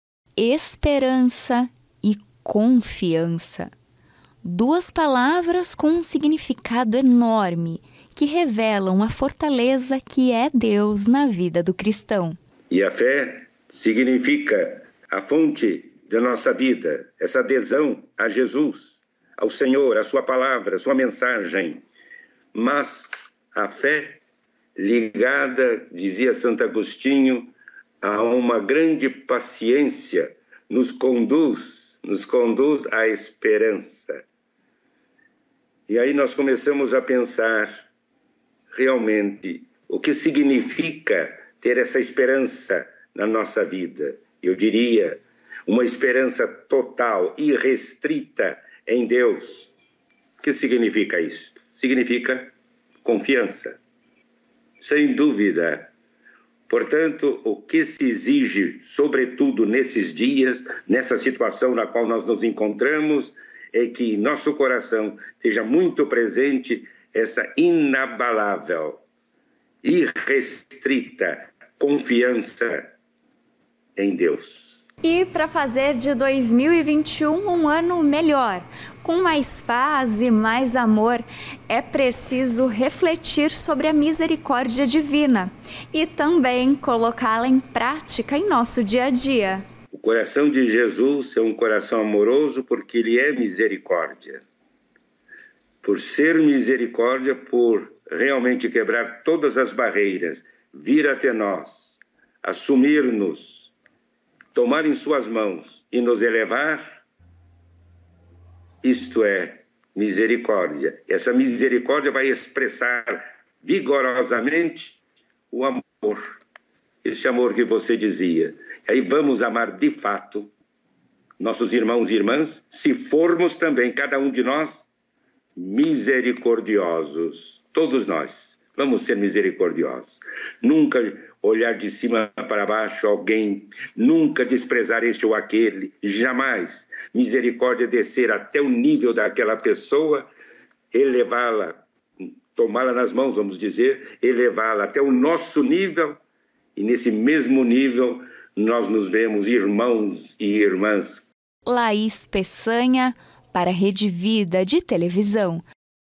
Dom Fernando Figueiredo reflete sobre como colocar a misericórdia de Deus em prática -JCTV- 18/01/21
Refletir sobre a misericórdia de Deus e colocá-la em prática pode fazer um 2021 melhor. A afirmação é do bispo emérito de Santo Amaro. Dom Fernando Figueiredo conversou com a jornalista Laís Peçanha.